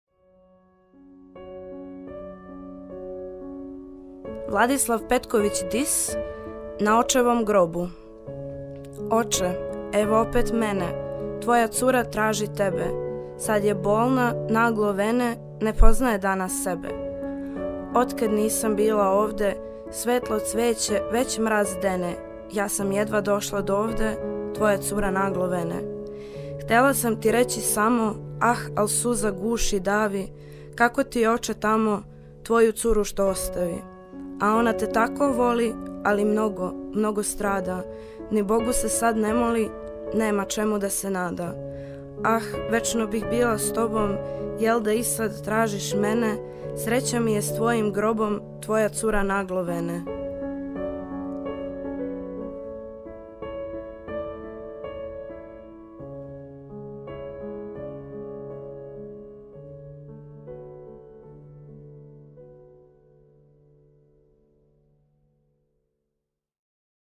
Vladislav Petković Dis - Na očevom grobu
Uživajte u pjesmi " Na očevom grobu" pjesnik Vladislav Petković Dis.
Poeziju govori Zoja Kovačević.